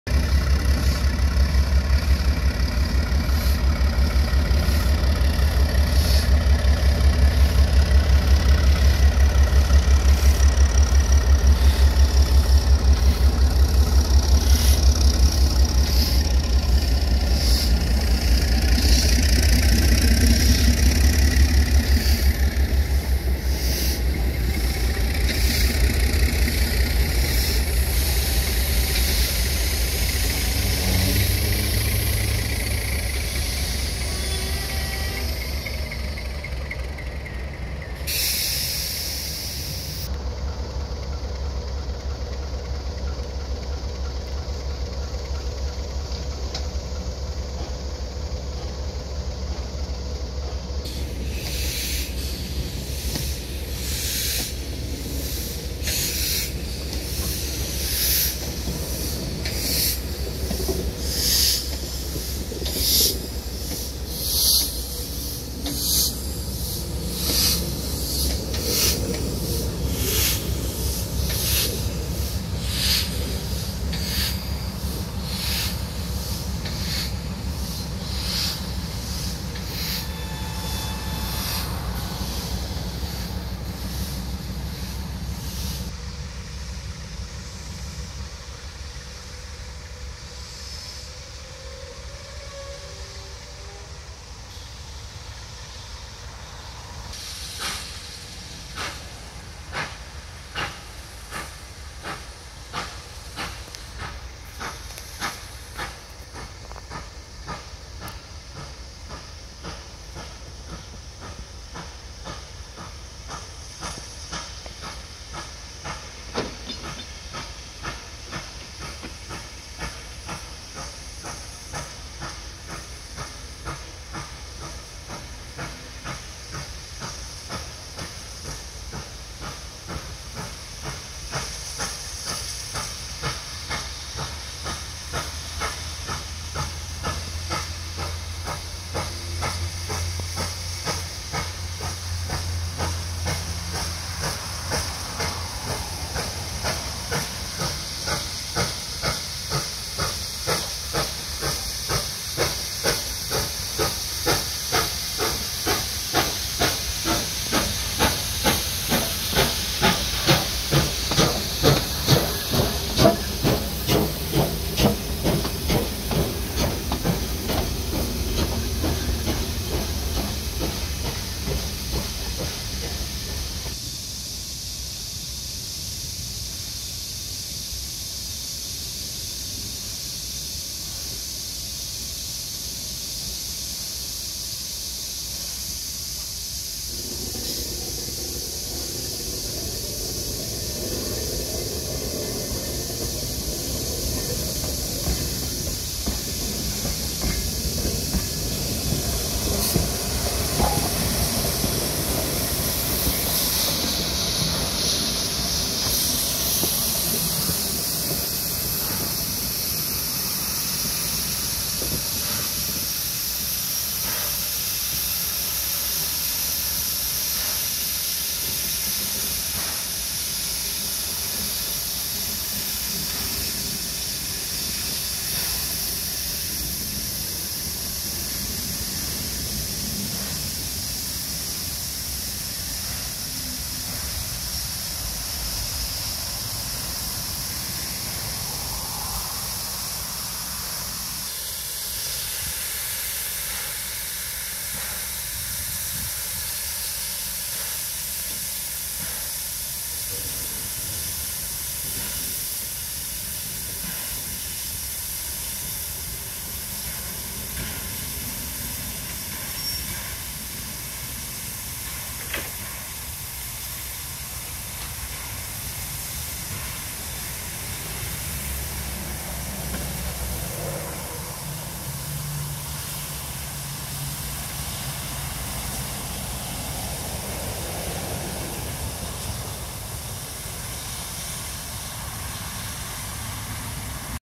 3801 in steam on the 24/2/21 short trial session after rework.
As 3801 takes a few short runs up and down the loop line diesle locomotive 4833 shunts 2705 and 3526 after they've had their fires lit on the afternoon of the 24/2/21 down on the arrival road to the roundhouse at Thirlmere railway museum 6029 was also in steam. All 4 locos would be having aparent boiler inspections the following day.